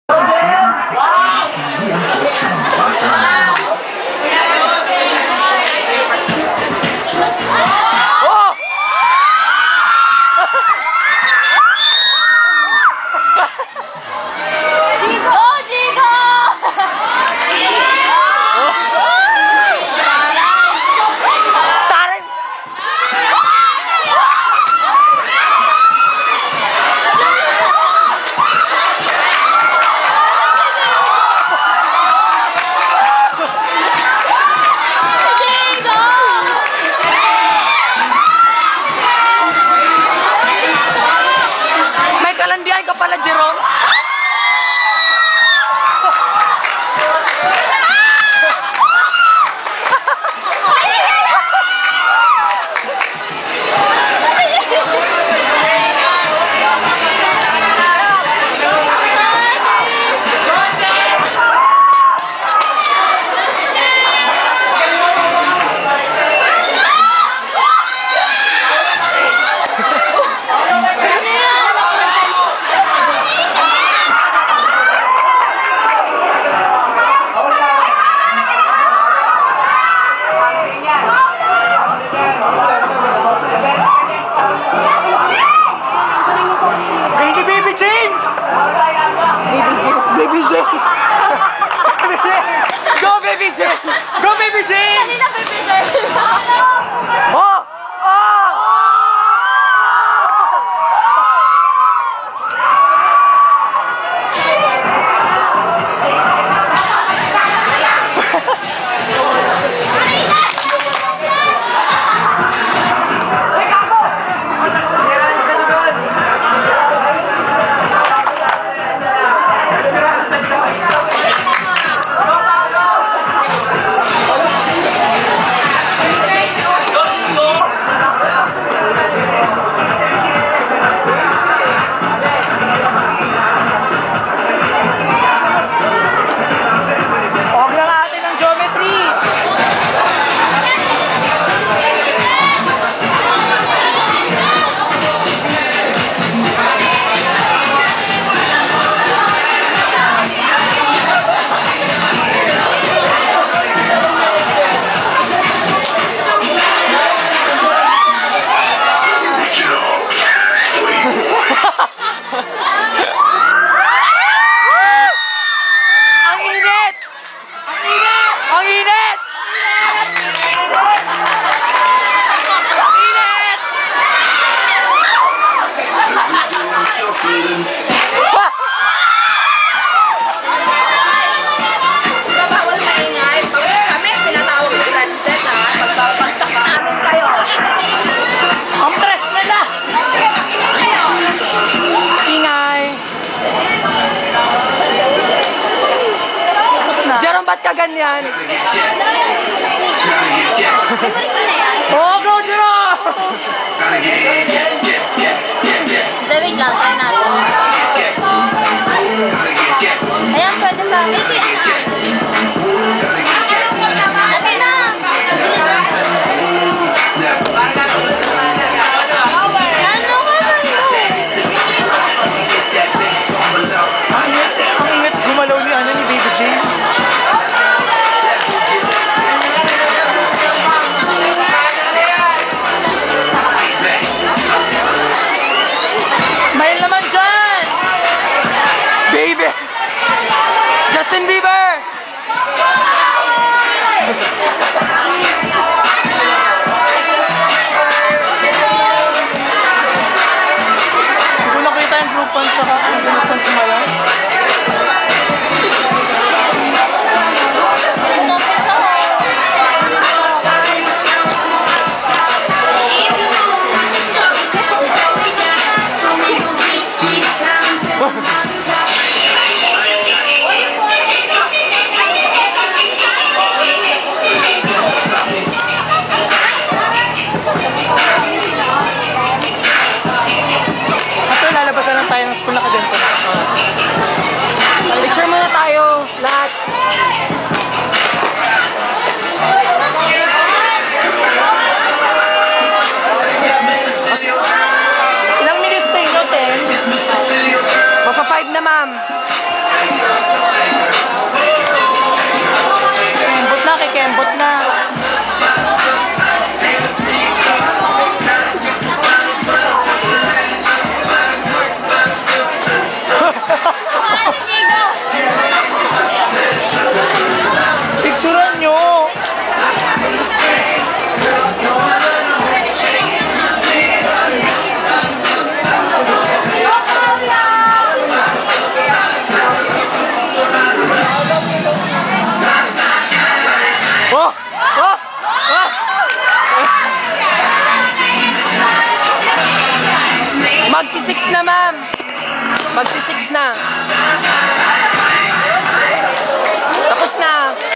Modern Dance : Group 5
Students from Rizal Technological University (CEIT-15-201P). A presentation for the Final Examination on our Physical Education 2.